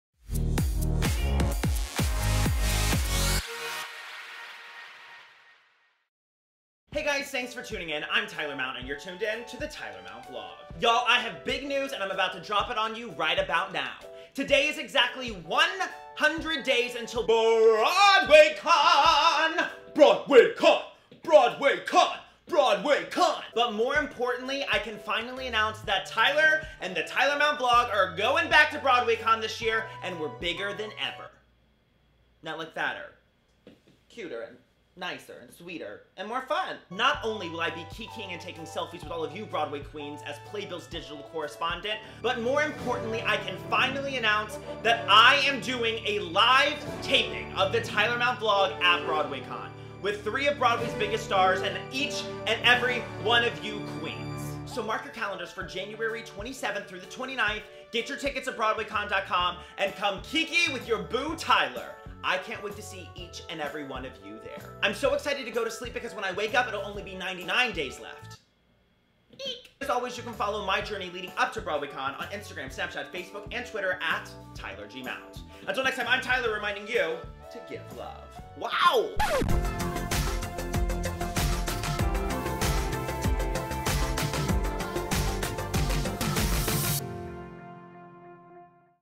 Big BroadwayCon News | TYLER MOUNT
The cat's out of the bag, and I'm comin' back to BroadwayCon 2017! Not only will I be on the floor as Playbill's Digital Correspondent, but you'll be able to kiki with in person at The Tyler Mount Vlog LIVE: Broadway Besties!

Tickets at: BroadwayCon.com

♦♦♦CONTACT TYLER♦♦♦